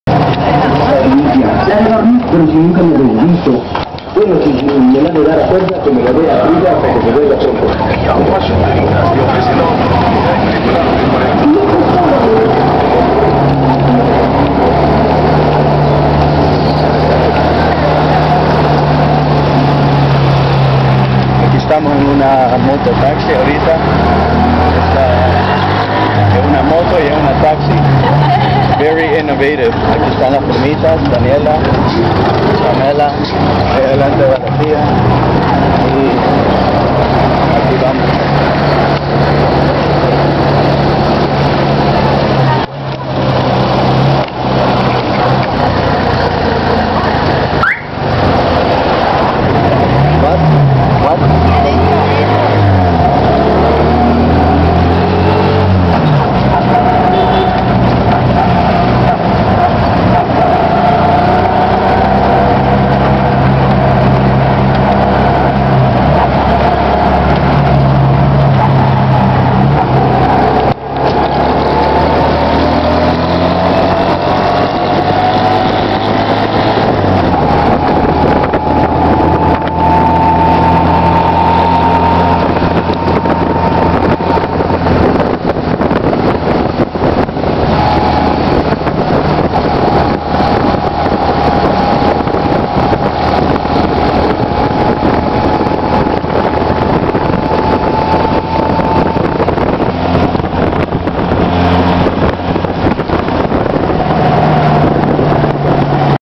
2009 | Moto Taxi ride in the streets of El Salvador.
Aqui estamos en un mototaxi. Perdimos la dignidad jajajjajajjaaa